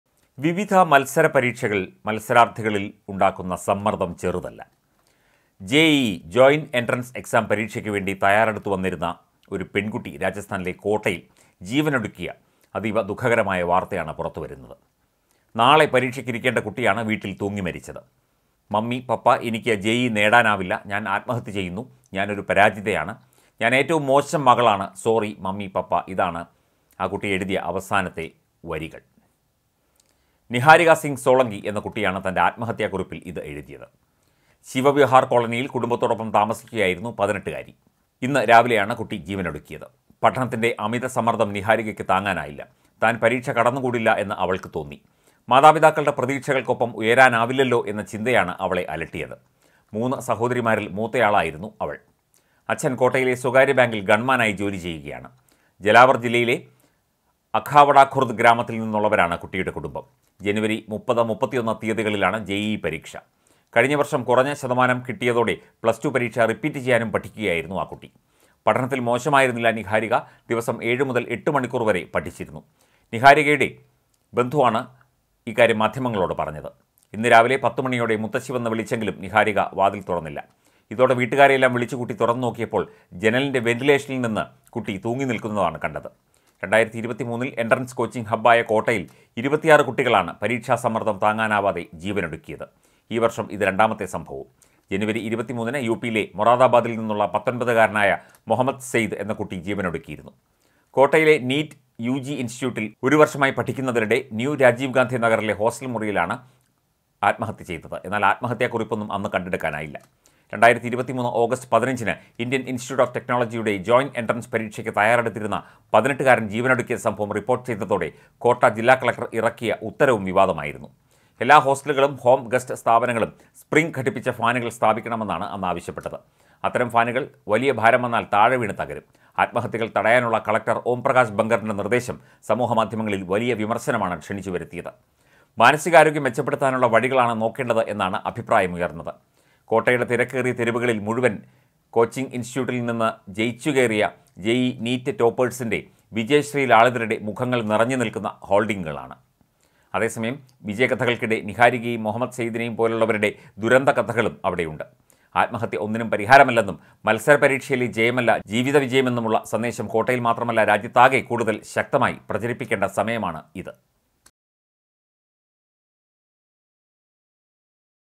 പരീക്ഷയിൽ തോൽക്കുമെന്ന് ആശങ്ക,,വിദ്യാർത്ഥിനി ജീവനൊടുക്കി | girl commits suicide at Kota
ജെ ഇ ഇ പരീക്ഷയ്ക്ക് വേണ്ടി തയ്യാറെടുത്തിരുന്ന പെൺകുട്ടി രാജസ്ഥാനിലെ കോട്ടയിൽ ജീവനൊടുക്കി. നാളെ പരീക്ഷയ്ക്ക് ഇരിക്കേണ്ട കുട്ടിയാണ് വീട്ടിൽ തൂങ്ങി മരിച്ചത്. | Mummy, Papa, I can't do JEE: girl commits suicide at Kota